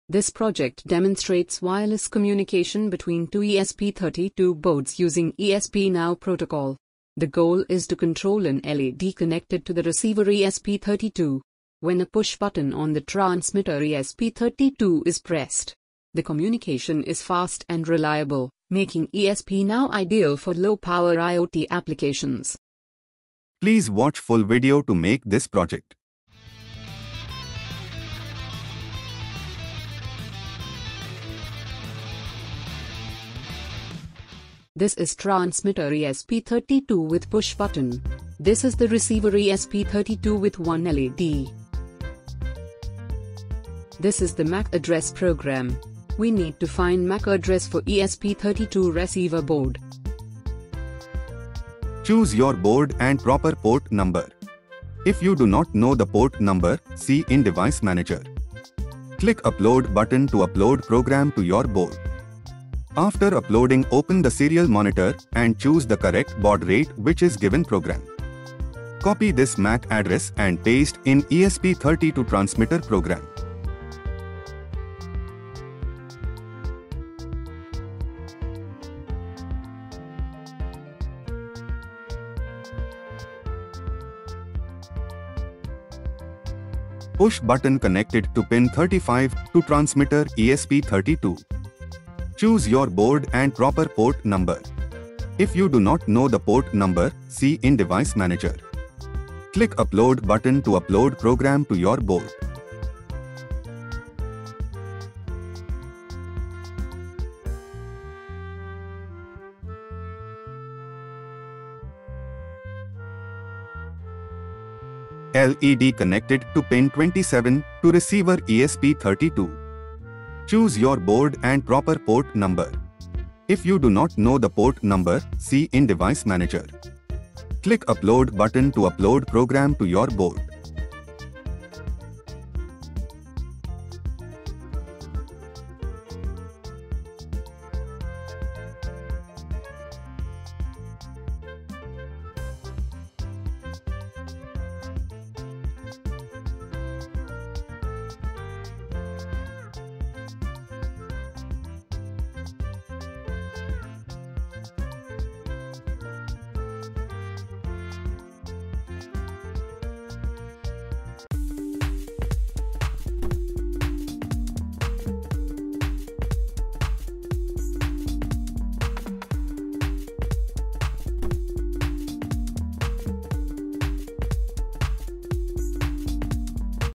ESP now protocol || esp-now examples || esp-now arduino || esp-now (esp32) || esp-now tutorial
Dear Viewers,

1. Introduction
This project demonstrates wireless communication between two ESP32 boards using ESP-NOW protocol. The goal is to control an LED connected to the receiver ESP32 (RX) when a push button on the transmitter ESP32 (TX) is pressed. The communication is fast and reliable, making ESP-NOW ideal for low-power IoT applications.

2. Objectives
Set up ESP-NOW communication between two ESP32 boards.
Transmit push button status from the TX side.
Control an LED on the RX side based on the button press.
3. Components Required
2 x ESP32 boards
1 x Push Button (for TX side)
1 x LED (for RX side)
1 x Resistor (220Ω for LED)
Jumper wires
Breadboard
Power supply or USB cable
4. ESP-NOW Protocol Overview
ESP-NOW is a wireless communication protocol developed by Espressif, allowing devices to send data directly without the need for Wi-Fi or Bluetooth connections. It’s highly efficient for IoT applications as it supports low-latency, one-to-many, or peer-to-peer communication.

4.1 Key Features
Low-latency communication (less than 1 millisecond)
Encrypted communication
Low power consumption
Supports one-to-many communication
5. Circuit Diagram
5.1 Transmitter (TX) Circuit
ESP32 connected to a push button.
Button is connected to a digital pin and ground, with an internal pull-up resistor used to detect button presses.
5.2 Receiver (RX) Circuit
ESP32 connected to an LED.
The LED is connected to a digital pin with a 220Ω resistor in series to limit current.